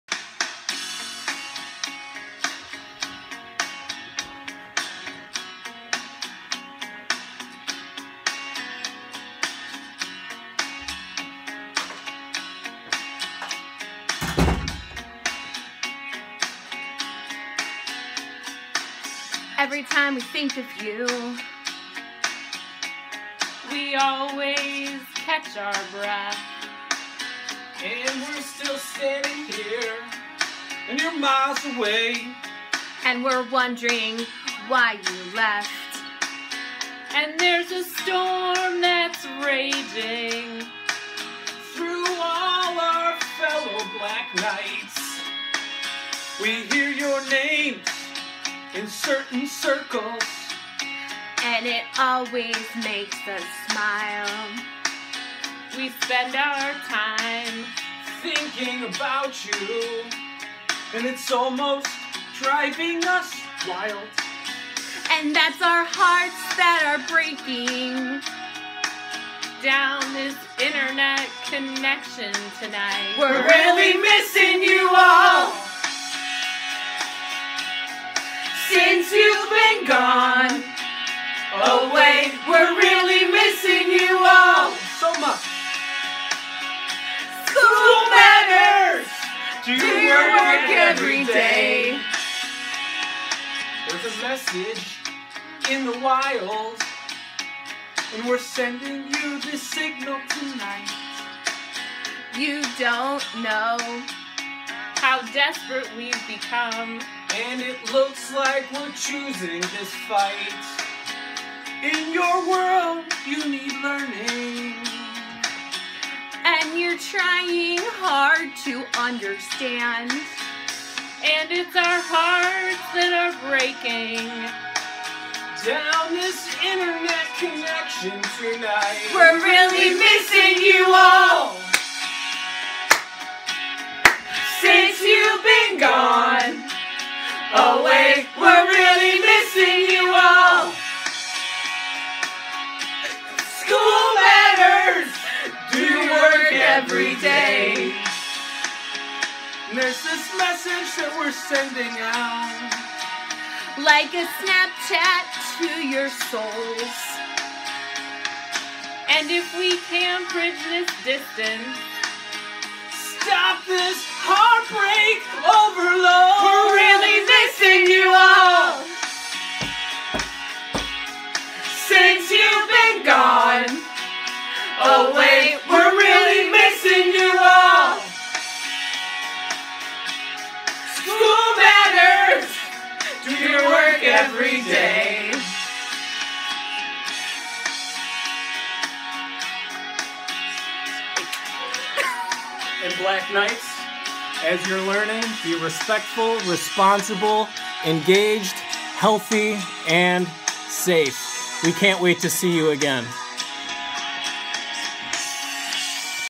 Missing You Marion Students!
The Building Leaders of Marion Central School District want their students to know that they are missed and that they should keep learning!